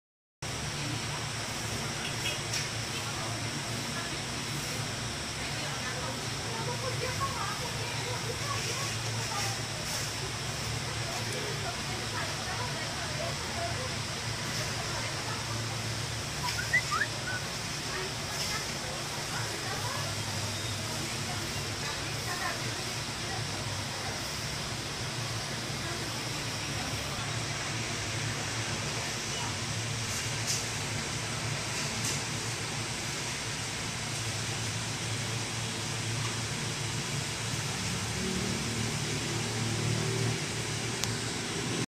Assalto ao Supermercado Cidade Sol
Dois elementos em uma bicicleta assaltaram por volta do meio dia de terça-feira (10/3/2015), o caixa Correspondente Bradesco do  Supermercado Cidade Sol, no largo São Francisco, bairro Joaquim Romão. A quantia subtraída pelos assaltantes não foi informada